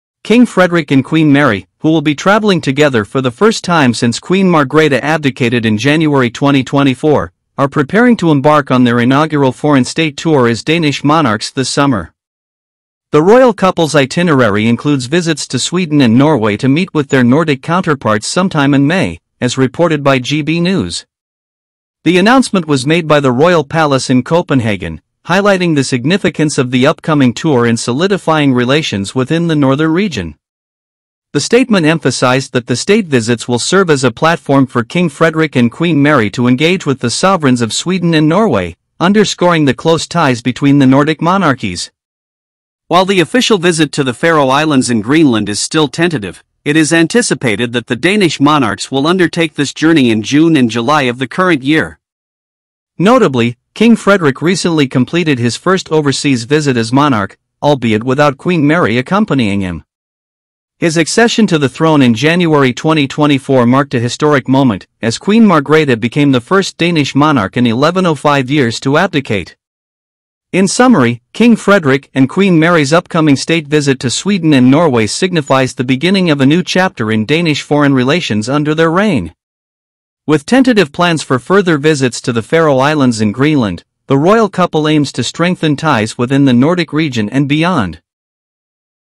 King Frederik and Queen Mary's First State Visit Announcement
👑 Exciting news from the Danish royal palace! King Frederik and Queen Mary are set to embark on their first state visit together, marking a historic moment since Queen Margrethe's abdication in January 2024.

In a statement released from Copenhagen, it was revealed that the royal couple will commence their journey in May, where they will visit Sweden and Norway to meet with fellow Nordic monarchs. This official visit not only signifies the beginning of their reign but also strengthens the bonds between the Nordic kingdoms.

While King Frederik recently undertook his first overseas visit solo, this upcoming tour will showcase the unity and diplomacy of the Danish monarchy under his reign.

Don't miss out on witnessing history in the making as King Frederik continues the legacy of his mother, Queen Margrethe, as the first Danish monarch to ascend the throne in over a millennium. 🌟🌍